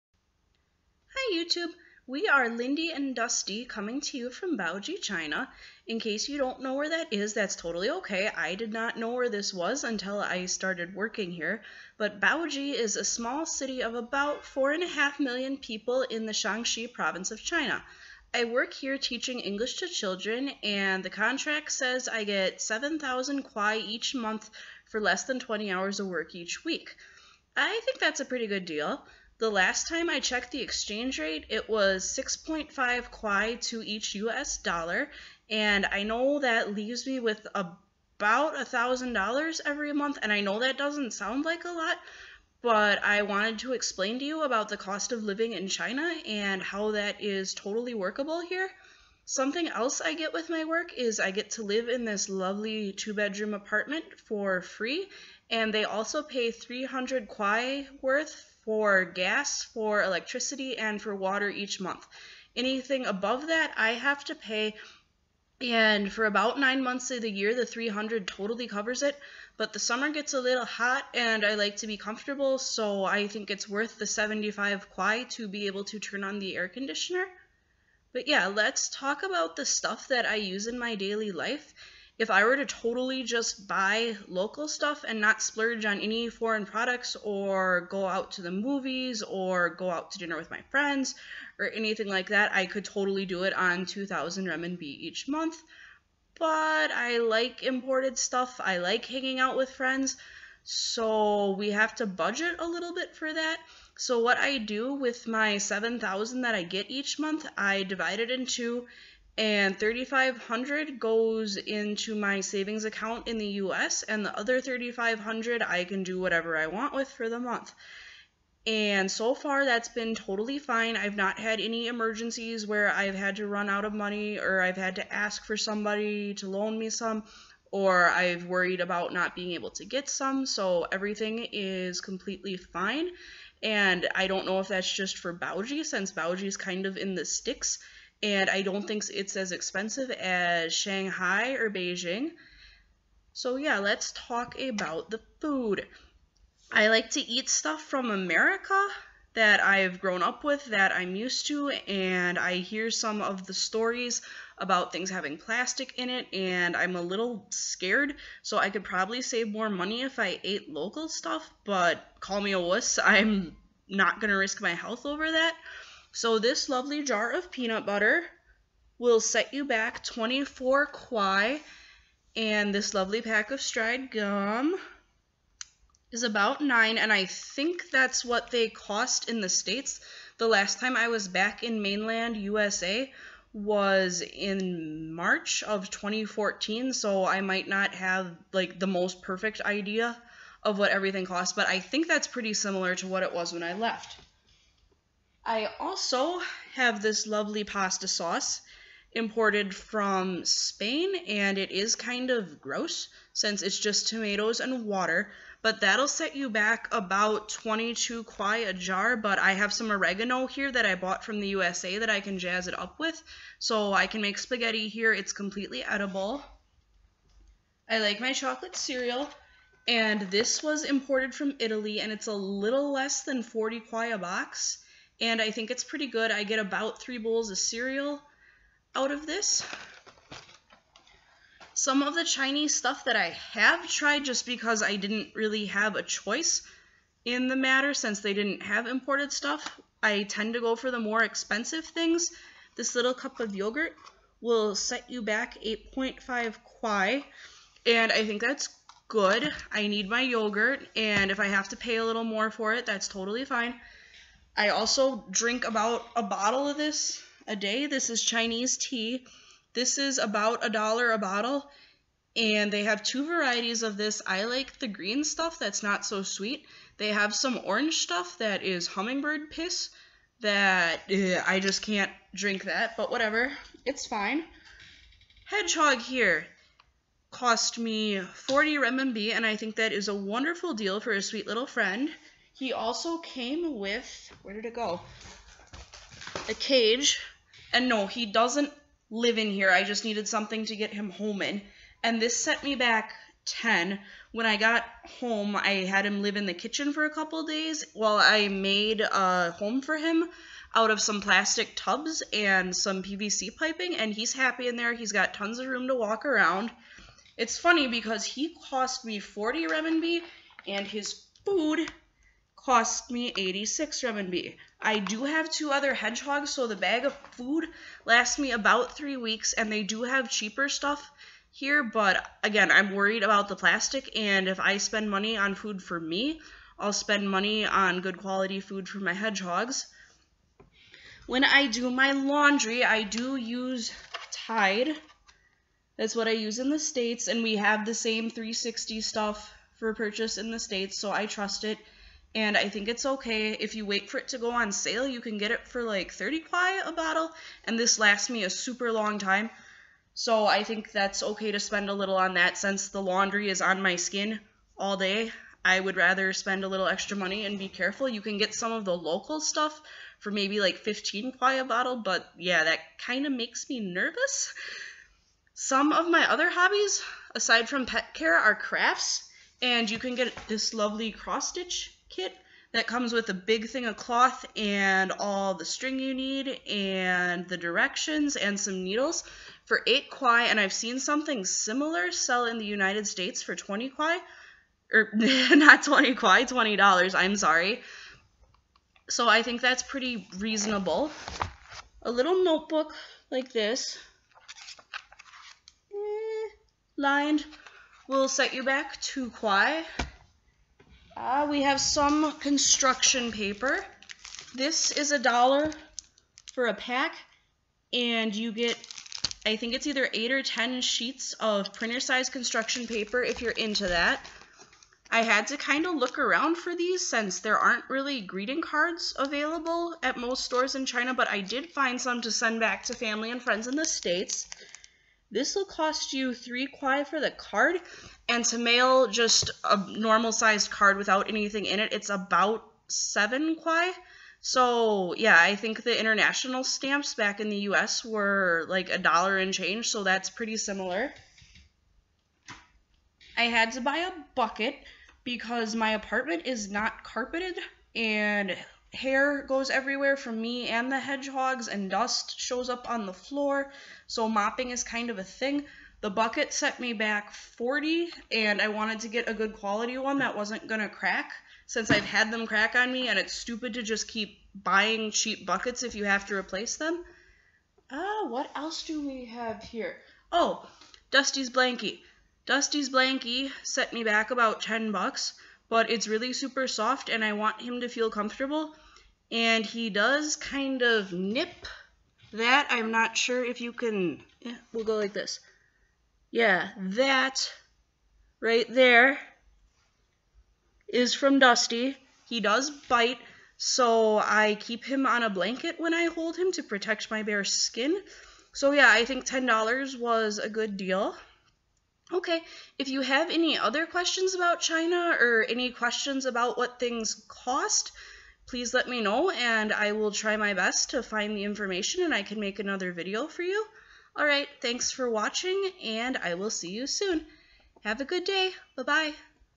Buying Stuff in Baoji
I thought I'd talk about the cost of living in small town China. I show some western products and how much they'll set you back as well as some other stuff that's local and the cost of those things as well.